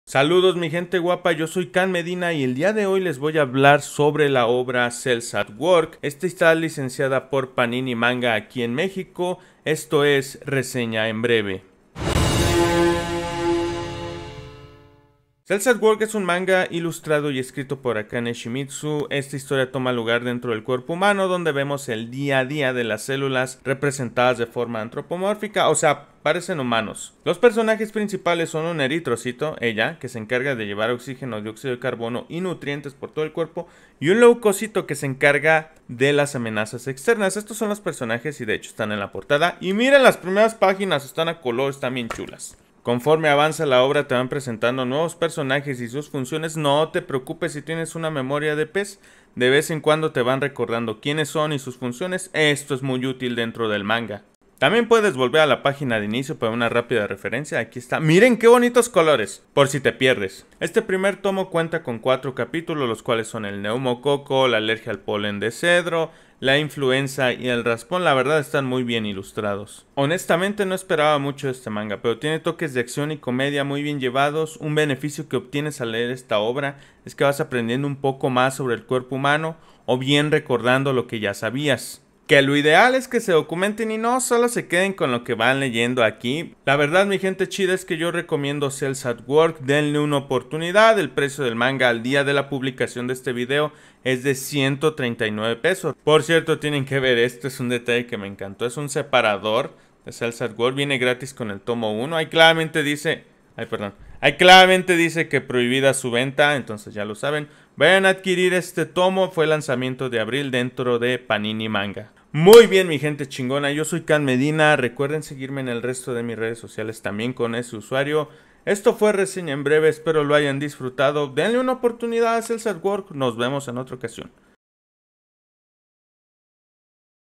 Reseña en Breve | Panini Manga | "Cells At Work"!
En 5 minutos o menos te diremos si "Cells At Work!" vale la pena o no. Esto es "Reseña en Breve".